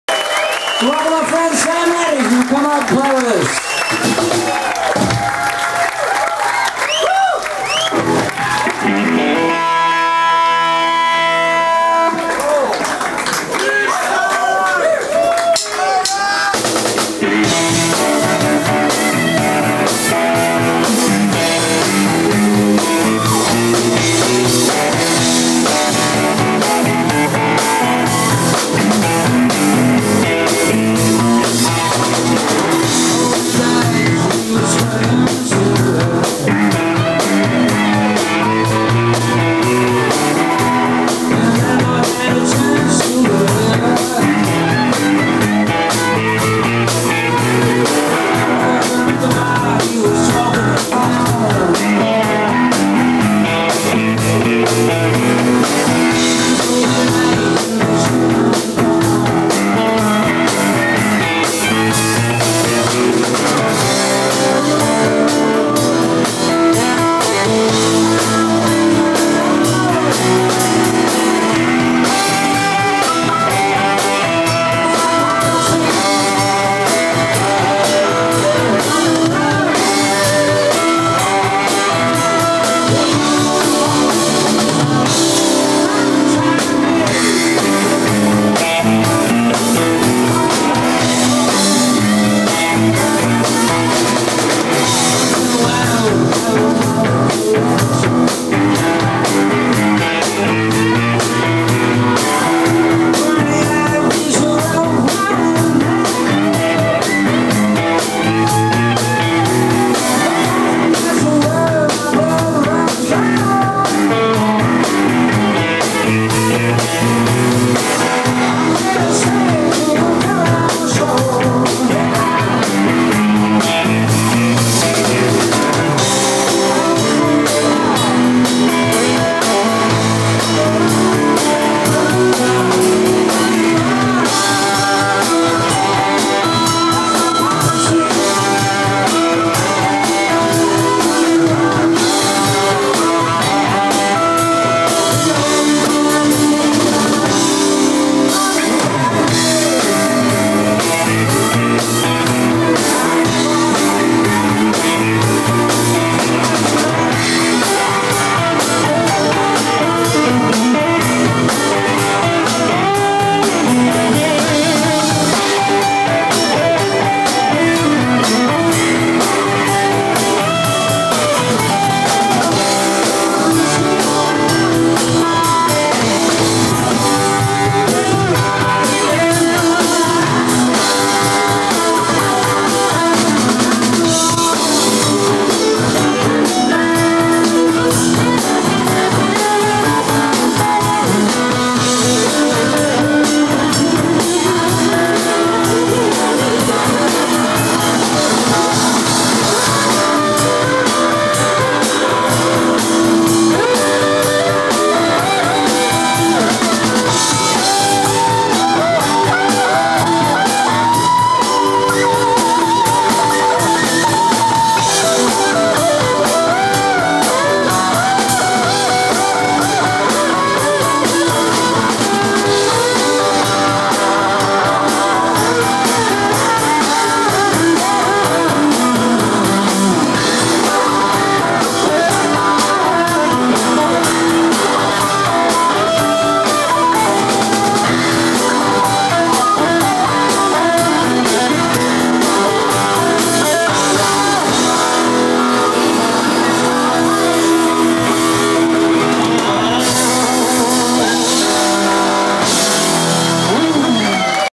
Nils Lofgren with Samuel Eddy - live Darmstadt 1995 - Underground Live TV
Nils Lofgren
with special guest:
Samuel Eddy  ...

check all releases of
these great two musician
Nils Lofgren and
Samuel Eddy

recorded live in
Darmstadt
Nieder-Ramstadt,
Steinbruch Theater
7.2.1995

only a rough
one camera
recording so not the
best quality but a
great performance
of Nils Lofgren
with guest musician
Samuel Eddy

or here:

oldUndergroundLiveTV

bLightTV